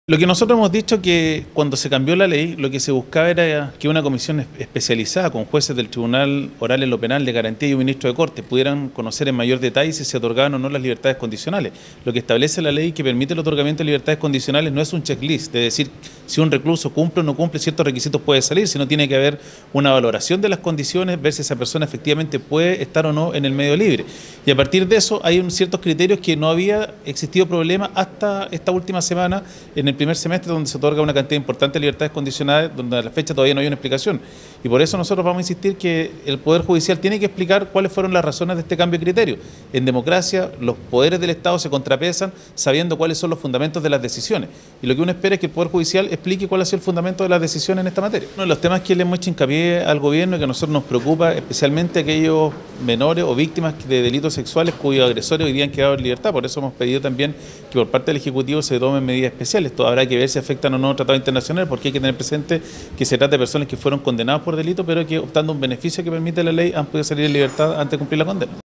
Presidente Comisión de Constitución exige explicaciones a Poder Judicial por libertad condicional
El senador Pedro Araya señaló que tras la abultada cantidad de reos beneficiados con la libertad condicional es necesario que el Poder Judicial explique los criterios que utilizó para ello, particularmente con los reos culpables de agresión sexual que quedaron en libertad.